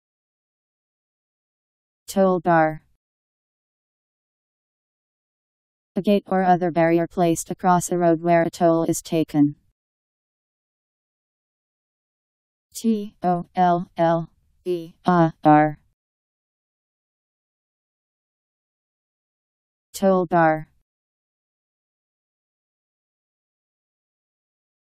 Toll-bar Meaning
Video shows what toll-bar means. a gate or other barrier placed across a road where a toll is taken.  Toll-bar Meaning. How to pronounce, definition audio dictionary. How to say toll-bar. Powered by MaryTTS, Wiktionary